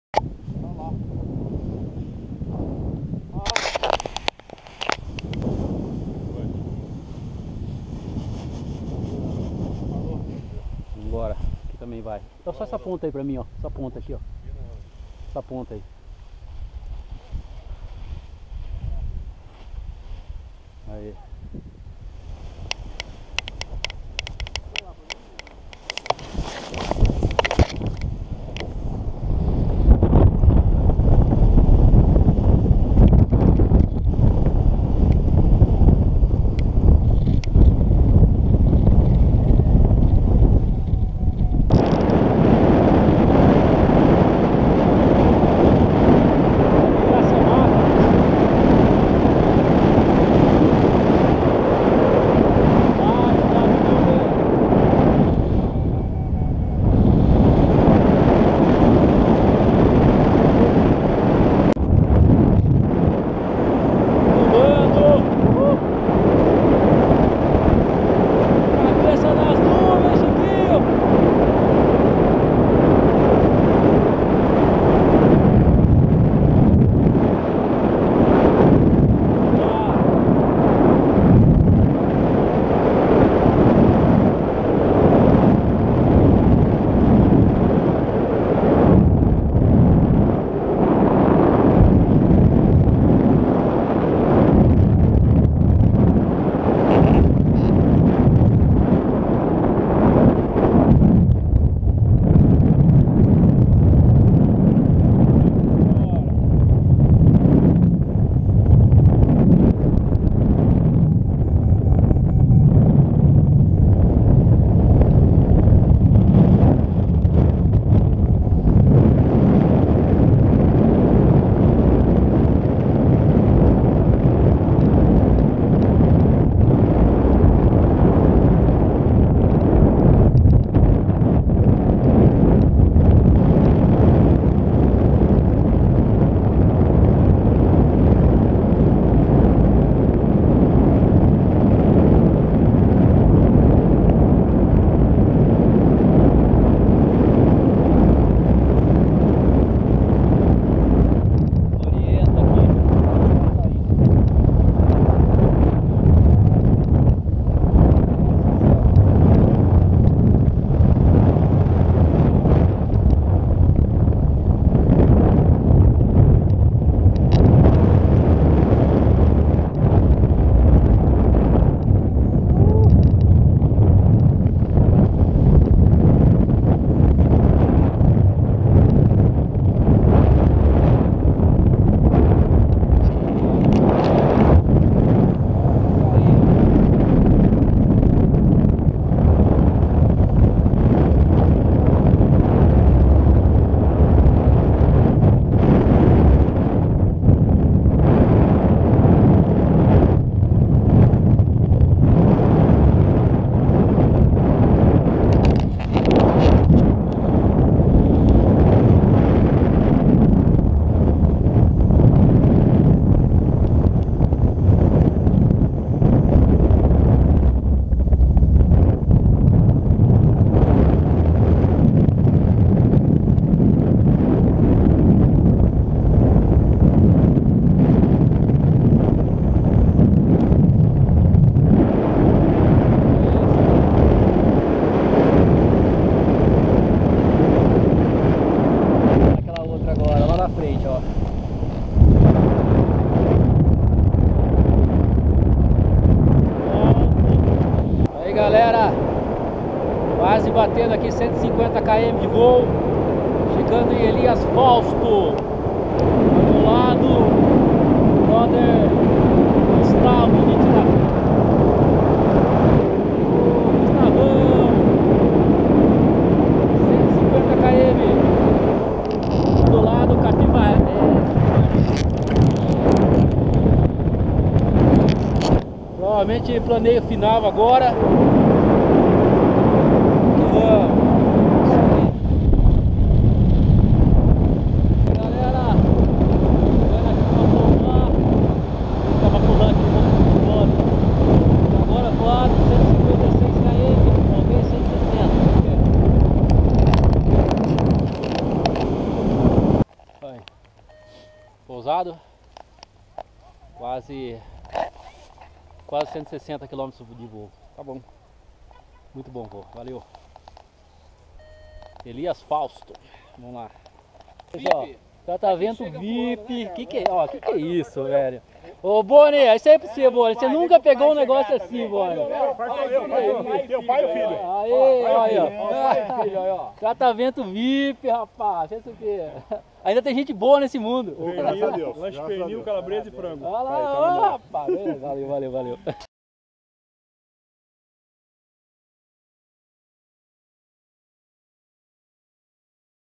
Voo Sta Rita Passa Quatro - 160km
Voo 160km  Rampa Espanhola em Sta Rita do Passa Quatro
Sta Rita - Elias Fausto